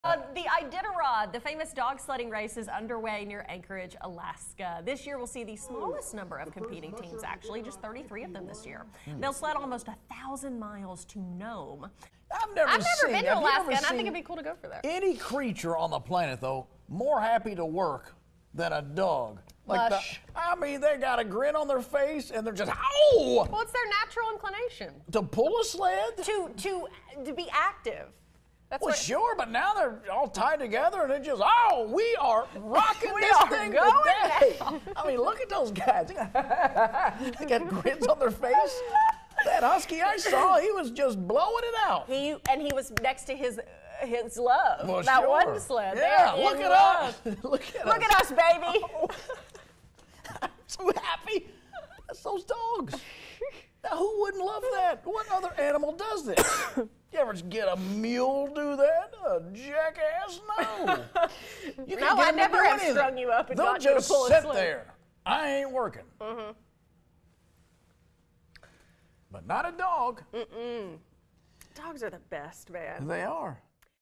Working Like A Dog
Lee and Hayley talk about how happy the dogs look to work at the Iditarod Trail Sled Dog Race.